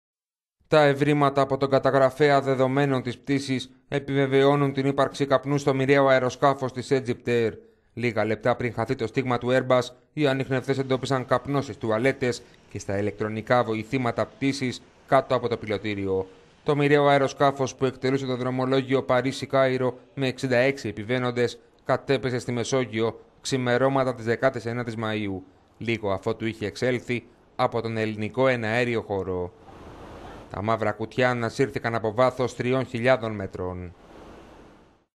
Egypt Air: Το «μαύρο κουτί» επιβεβαίωσε τους καπνούς στο μοιραίο αεροσκάφος
Τα πρώτα ευρήματα από τον καταγραφέα δεδομένων της πτήσης επιβεβαιώνουν την ύπαρξη καπνού στο μοιραίο αεροσκάφος της Egypt Air.

Λίγα λεπτά πριν χαθεί το στίγμα του Airbus oι ανιχνευτές εντόπισαν καπνό στις τουαλέτες και στα ηλεκτρονικά βοηθήματα πτήσης κάτω από το πιλοτήριο. Τα δεδομένα από το «μαύρο κουτί» συμπίπτουν με τα ηλεκτρονικά μηνύματα που είχε εκπέμψει το αεροσκάφος λίγο πριν την πτώση του.

Το μοιραίο αεροσκάφος που εκτελούσε το δρομολόγιο Παρίσι-Κάιρο με 66 επιβαίνοντες κατέπεσε σ…